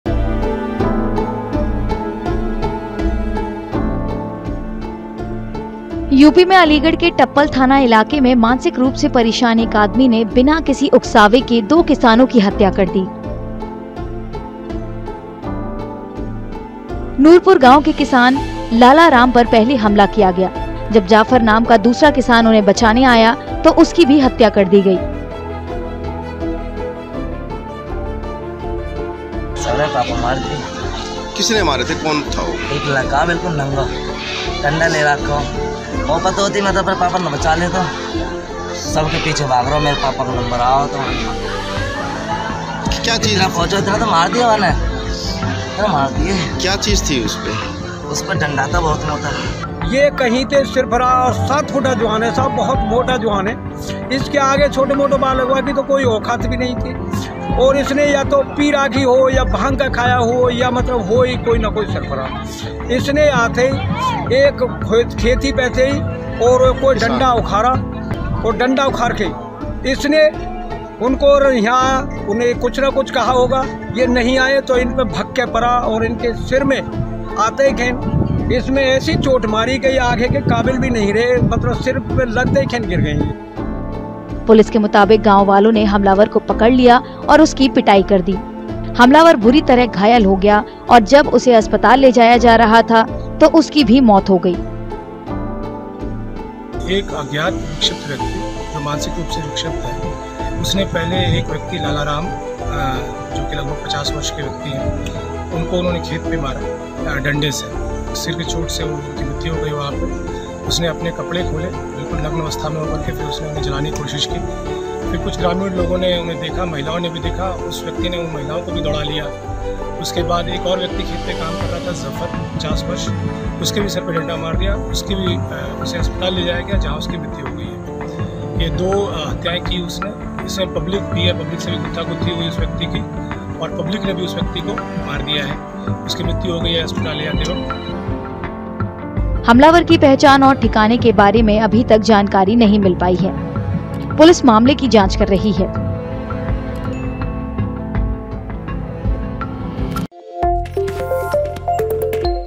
अलीगढ़ जिले में मानसिक रूप से बीमार आदमी ने दो किसानों की हत्या की
यूपी में अलीगढ़ के टप्पल थाना इलाके में मानसिक रूप से परेशान एक आदमी ने बिना किसी उकसावे के दो किसानों की हत्या कर दी. नूरपुर गांव के किसान लालाराम पर पहले हमला किया गया, जब जाफर नाम का दूसरा किसान उन्हें बचाने आया, तो उसकी भी हत्या कर दी गई.